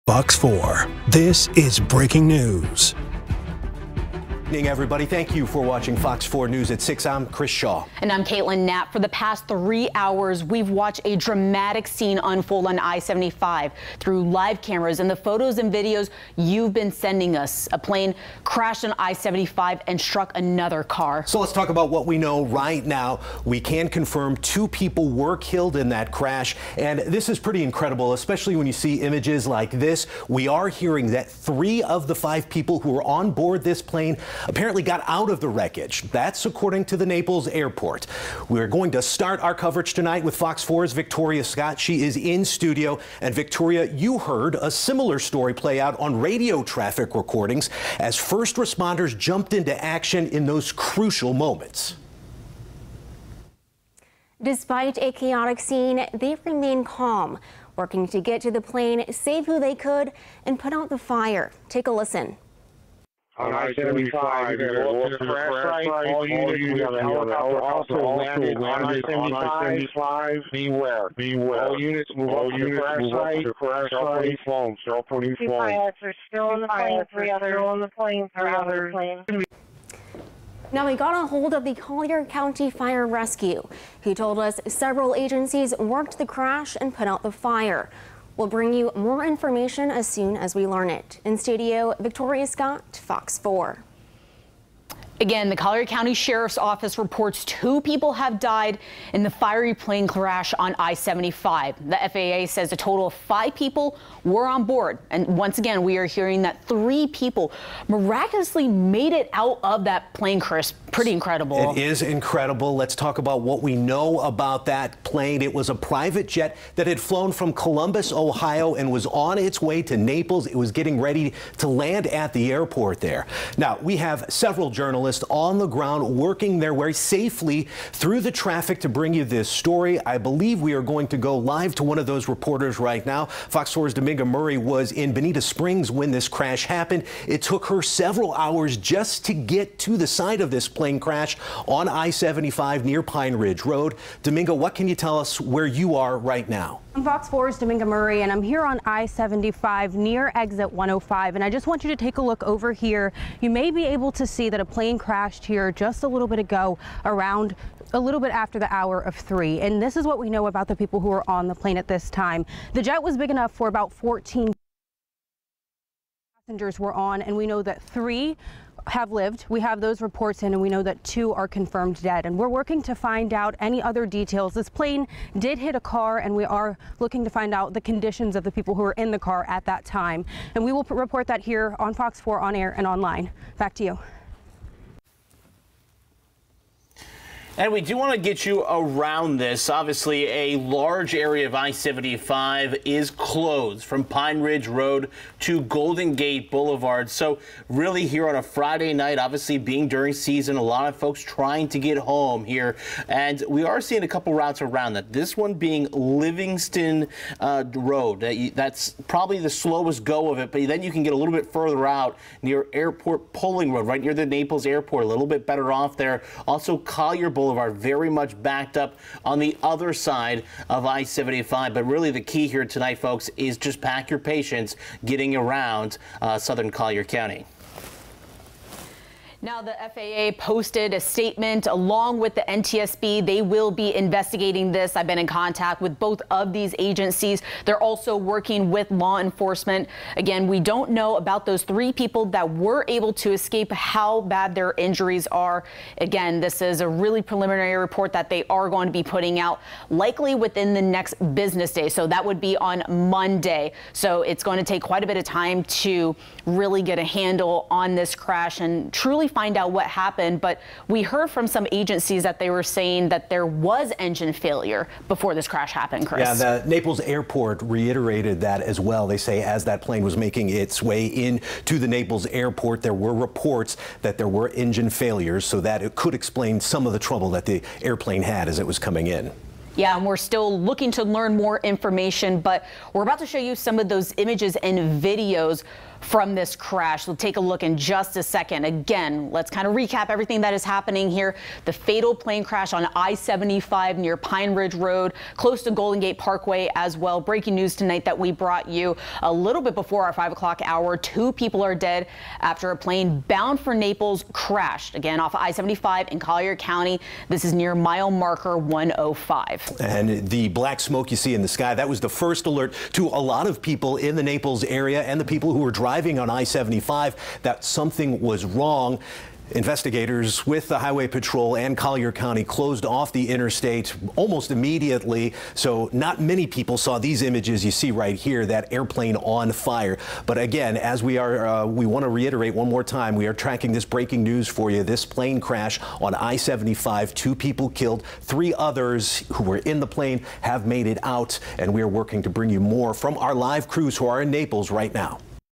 6PM Update on Naples Plane Crash: There were survivors
According to the Naples Airport, three of the five on board escaped the wreckage. Two are confirmed dead.